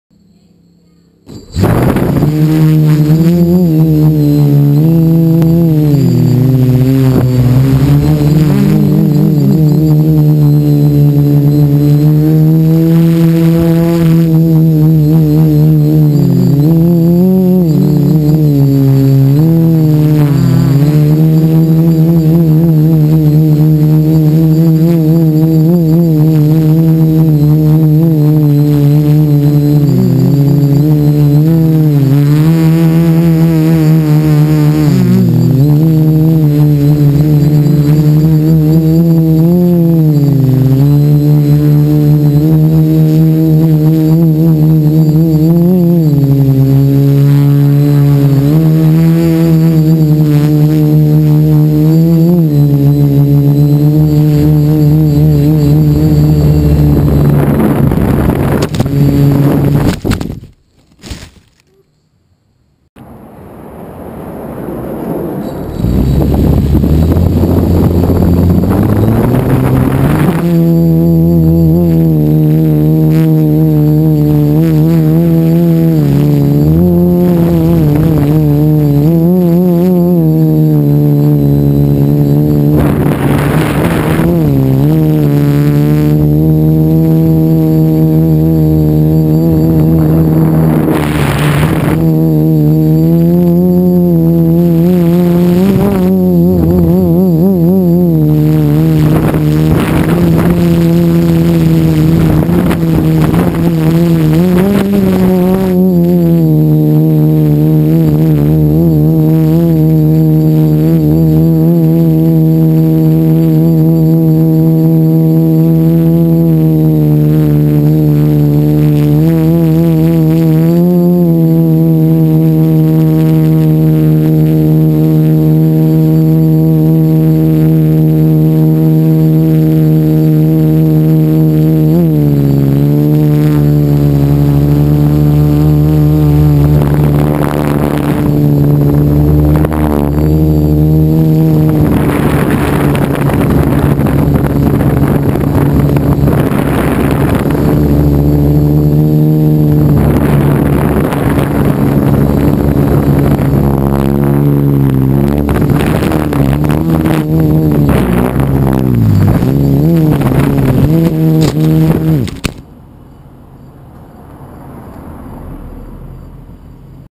Drone stable with GPS mode
I was very happy with this flight even though I broke a quadcopter arm!